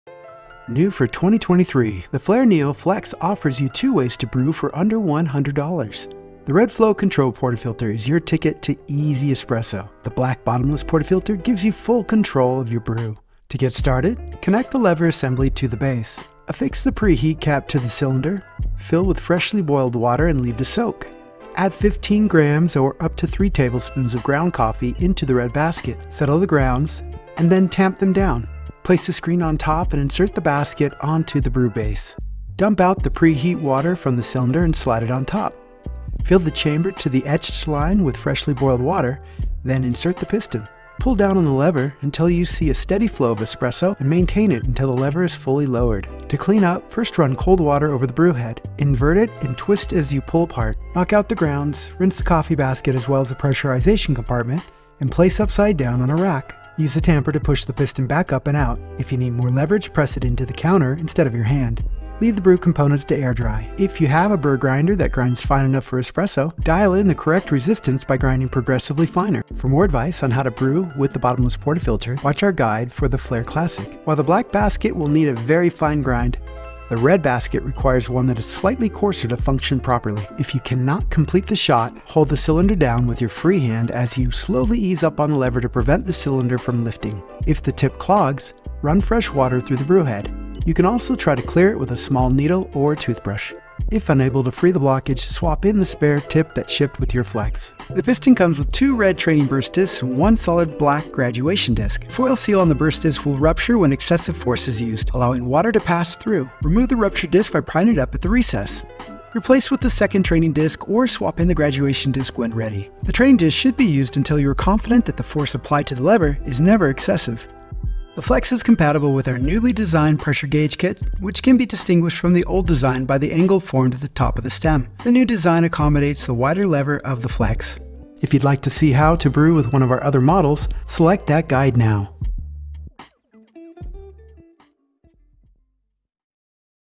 Flair Espresso Maker NEO Flex | 濾壓指南 (Flair Espresso Maker NEO Flex Brewing Guide)
歡迎收看Flair Espresso Maker NEO Flex的濾壓指南，你可以在這個影片中看到我們對NEO Flex濾壓流程的說明，以及對問題及對策的講解。

想獲得更多NEO Flex相關資訊，請訪問我們台灣的網站